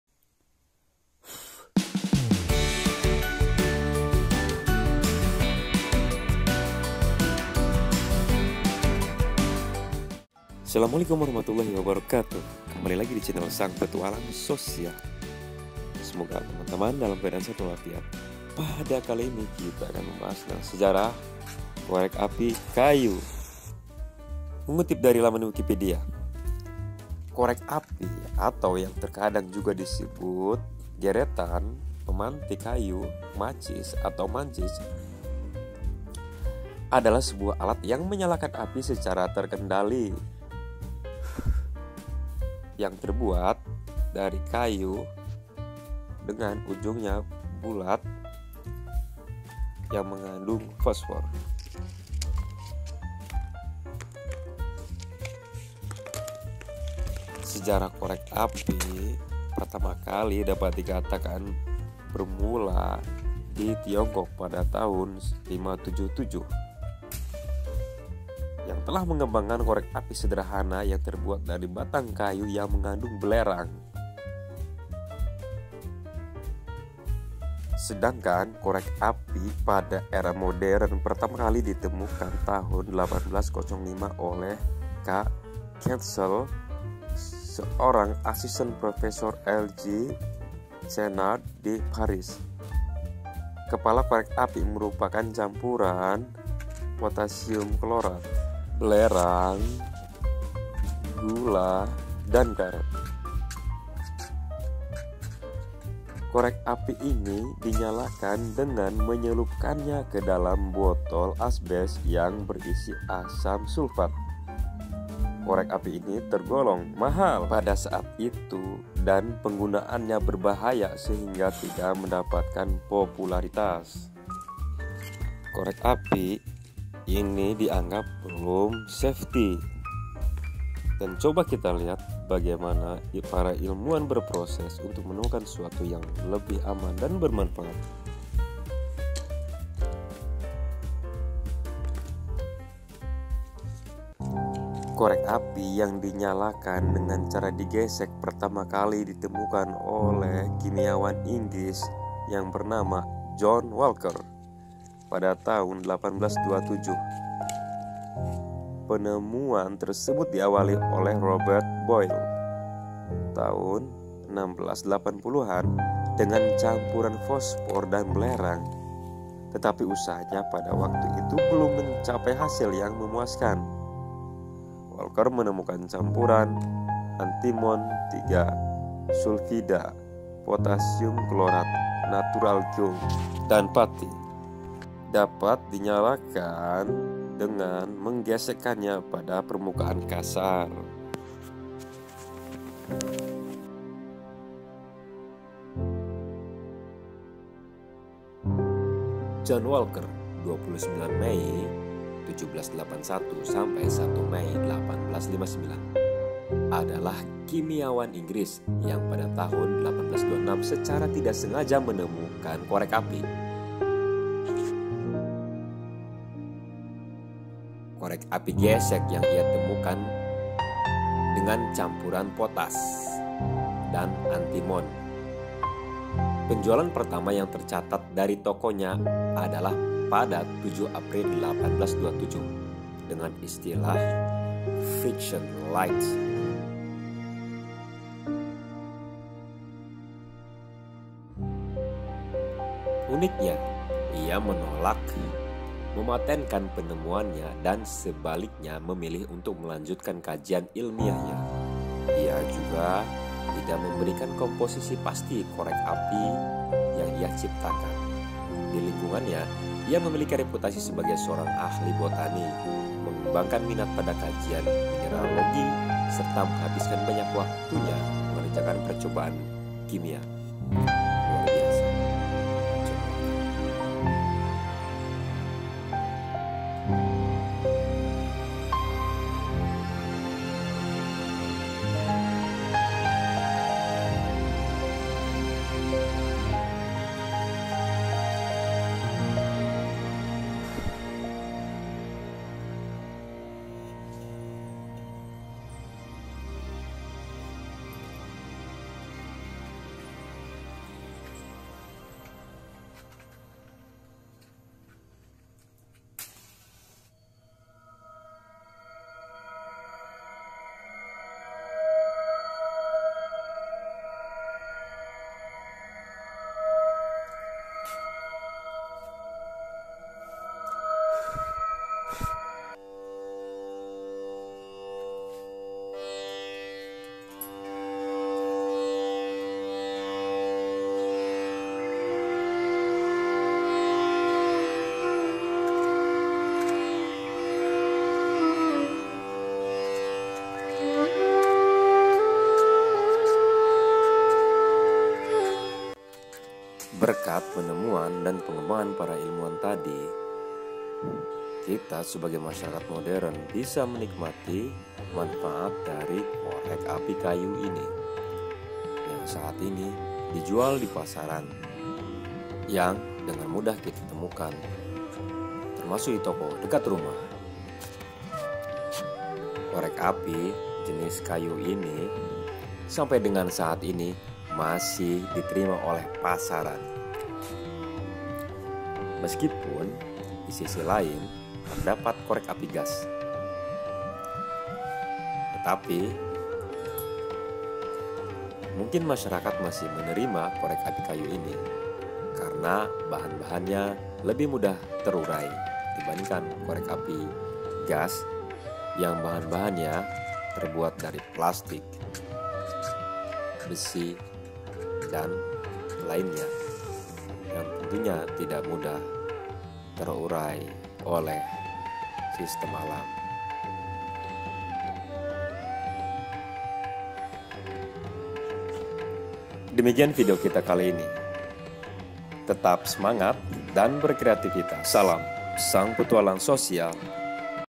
Begini Awal Mula Korek Api Kayu
Mengurai sejarah korek api kayu yang hari ini nasih kita gunakan dan sangat bermanfaat.
saksikan lengkap videonya

Salam
Sang Petualang Sosial